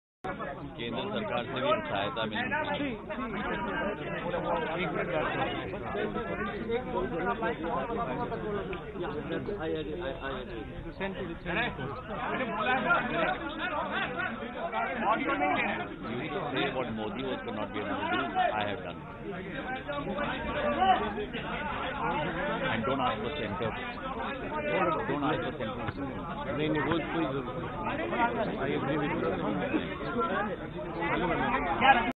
Cong leaders advising party president what to say before media
A man who can’t speak for 2 minutes without tuition wants to be India’s Prime Minister...